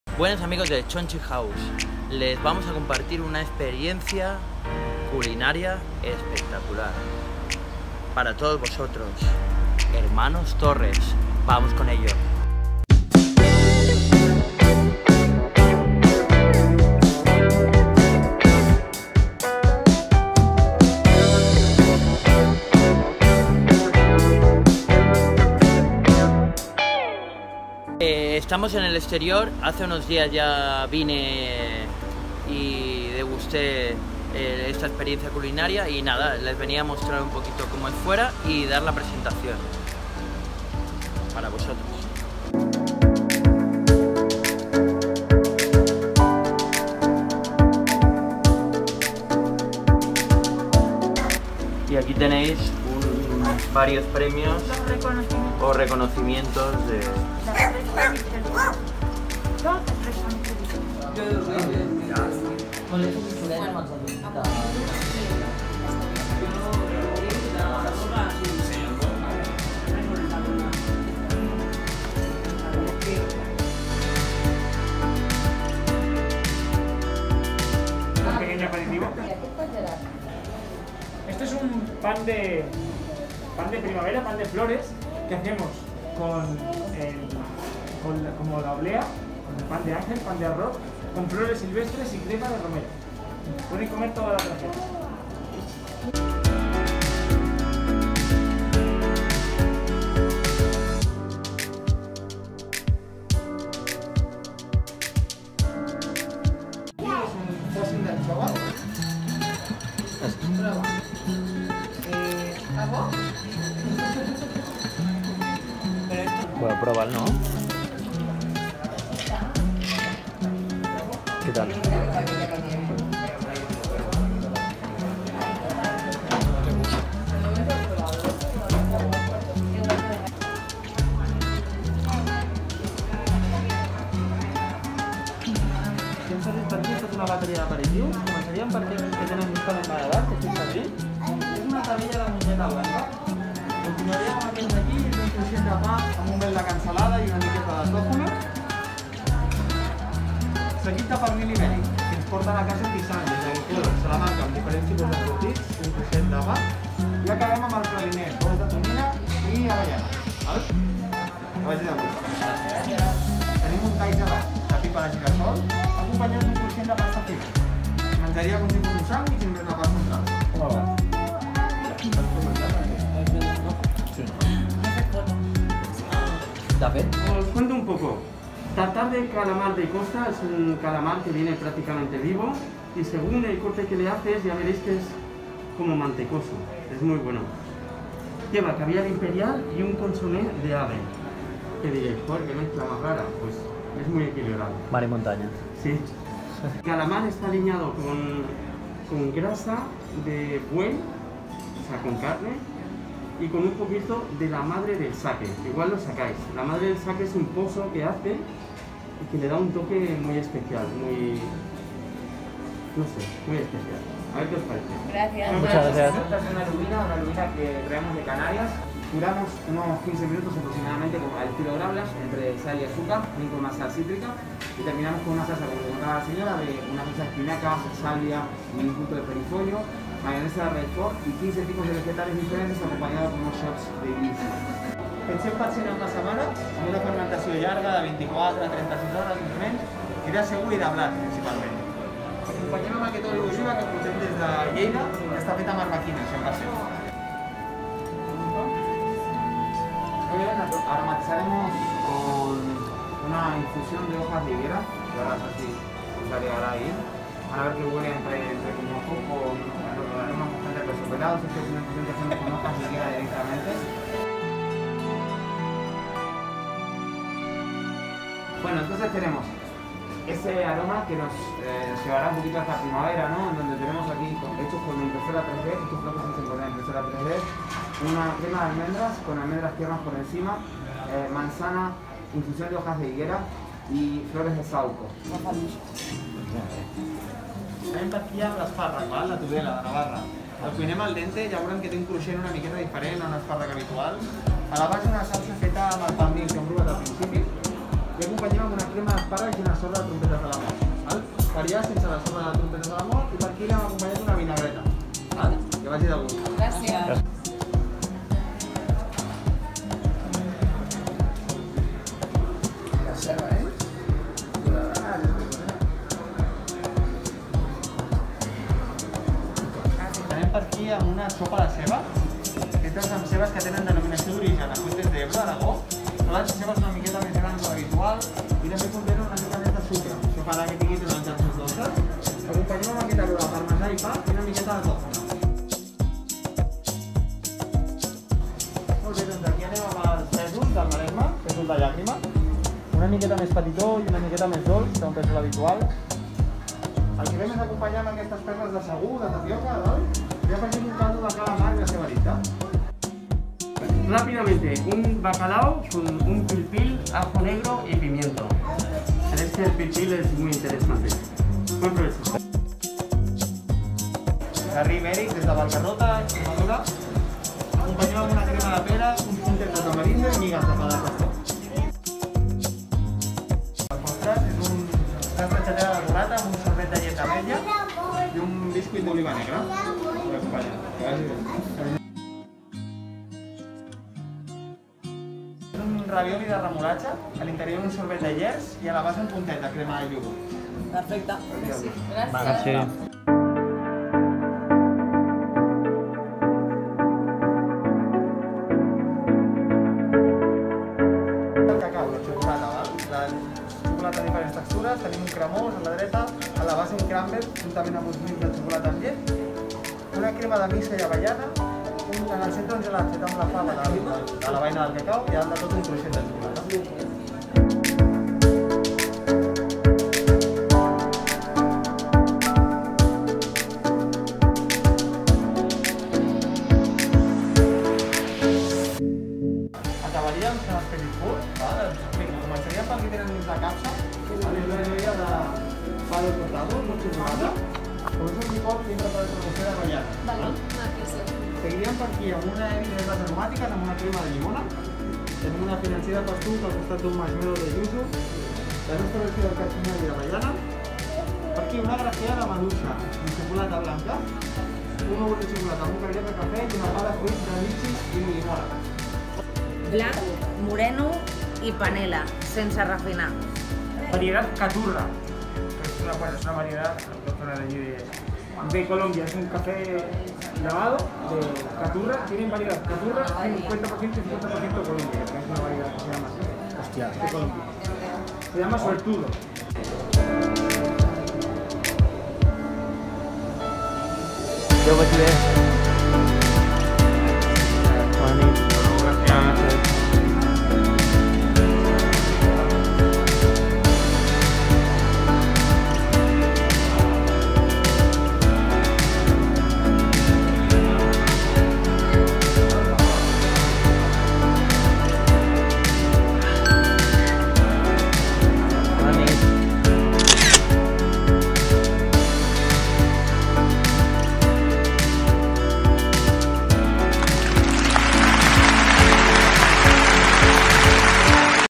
COCINA Hermanos Torres, Dos estrellas Michelin, una experiencia culinaria de primer nivel 👍
Hola amigos aqui les comparto un video, de mi experiencia en el restaurante "Cocina, Hermanos Torres" espero que les guste el video.

Amigos también hemos visitado hace tiempo el restaurante con tres estrellas michelin "Azurmedi"
os dejo los enlaces: